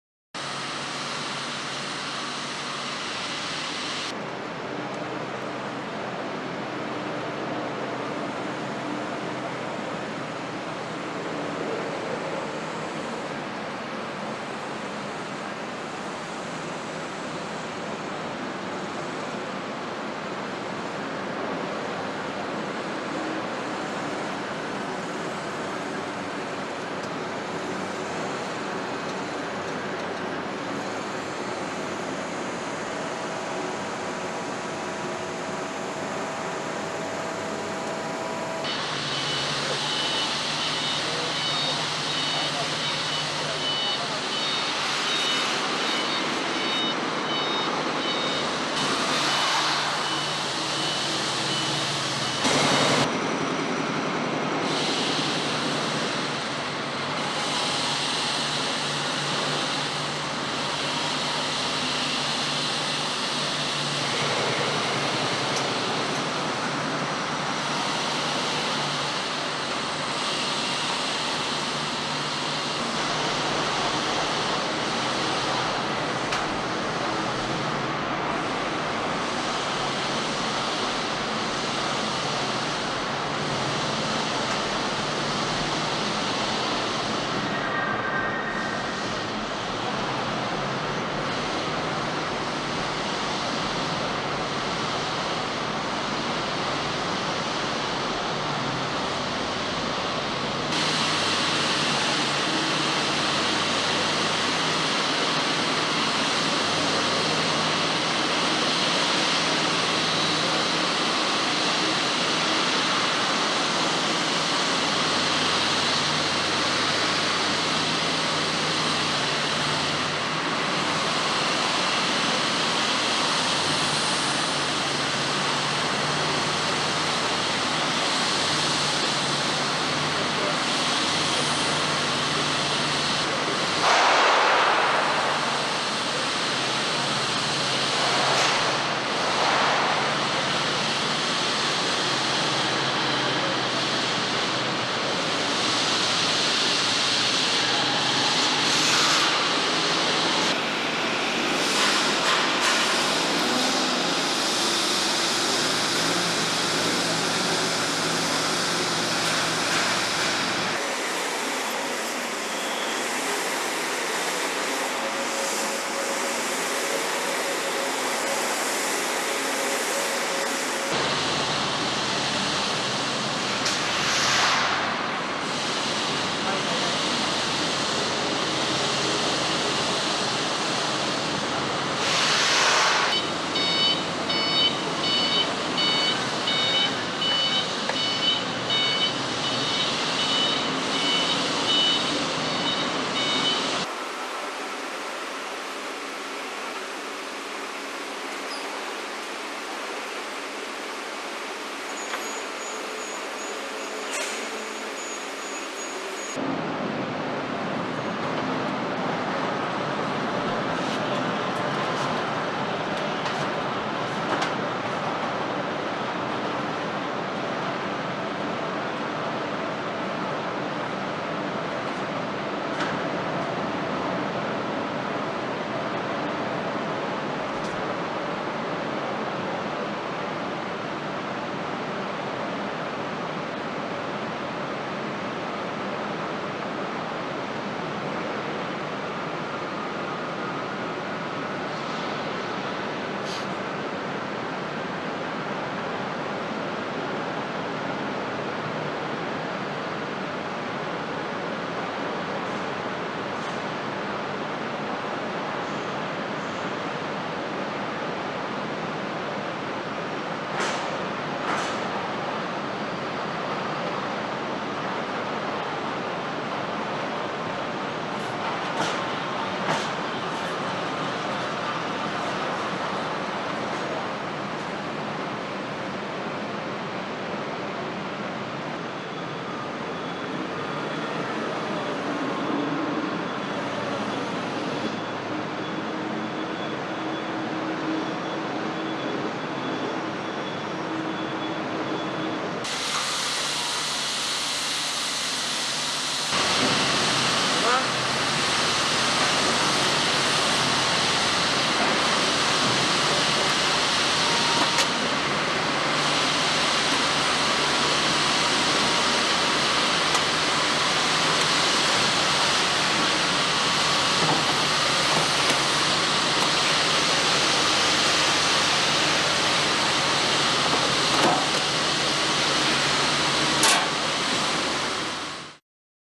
Storax Ranger
Video showing the Ranger pallet shuttle in operation.
The Ranger stores and retrieves pallets in a high density rack structure; Eliminating rack damage and increasing warehouse productivity